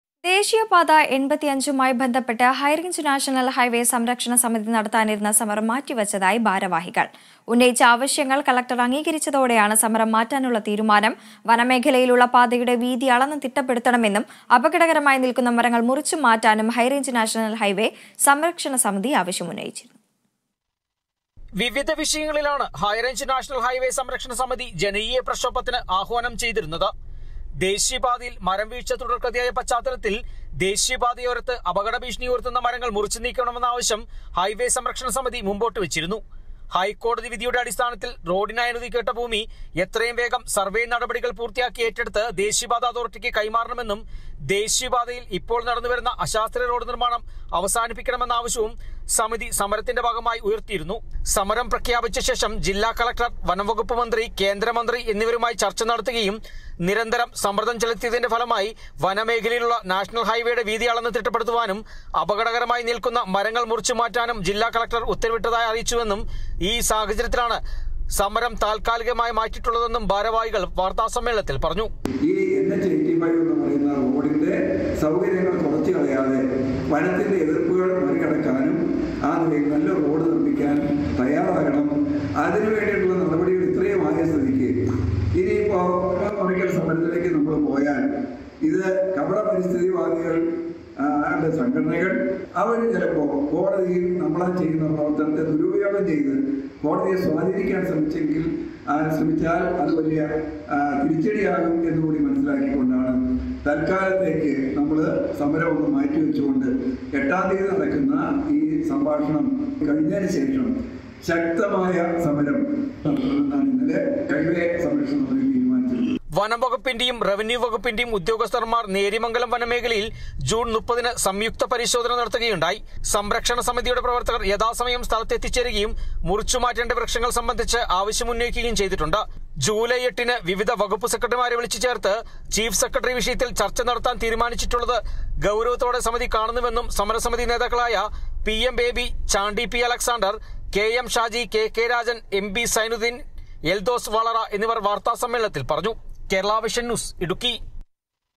ഹൈറേഞ്ച് നാഷണല്‍ ഹൈവേ സംരക്ഷണ സമിതി നടത്താനിരുന്ന സമരം മാറ്റിവച്ചതായി ഭാരവാഹികള്‍
ദേശീയപാത എണ്‍പത്തിയഞ്ചുമായി ബന്ധപ്പെട്ട്  ഹൈറേഞ്ച് നാഷണല്‍ ഹൈവേ സംരക്ഷണ സമിതി  നടത്താനിരുന്ന സമരം മാറ്റിവച്ചതായി ഭാരവാഹികള്‍. ഉന്നയിച്ച ആവശ്യങ്ങള്‍ കളക്ടര്‍ അംഗീകരിച്ചതോടെയാണ് സമരം മാറ്റാനുള്ള തീരുമാനം

About COA